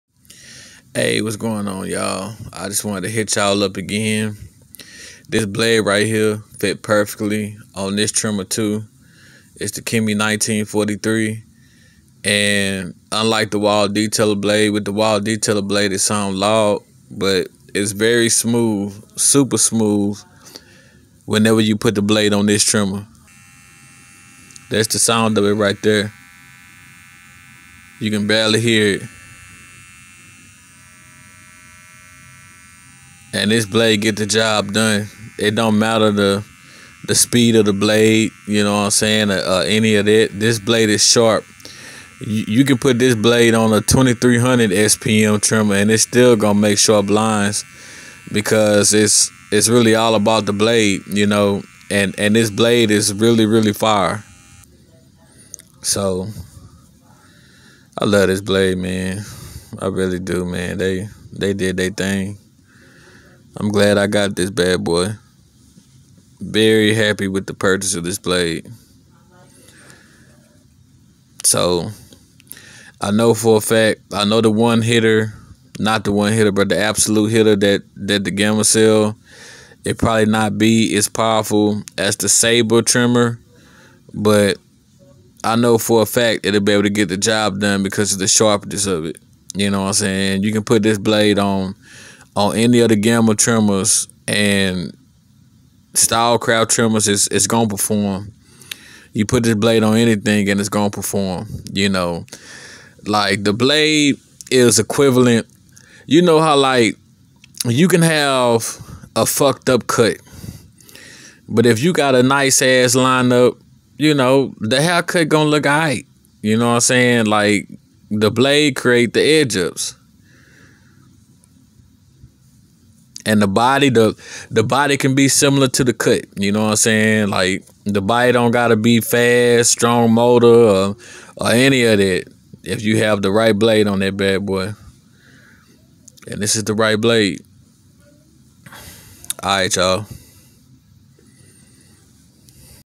stylecraft the one precision blade on the kemei 1943
Its so easy lining up the corners of your hair with this blade.